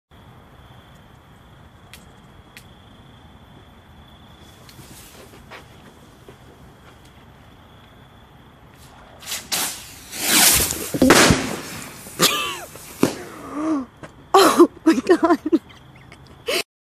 Mortars in the Water
Two of us are standing on the dock about a foot apart. We throw the mortar in the water & SWOOSH!!! It comes straight for us & goes right in between us & lands behind us. Never touched either of us. Whew.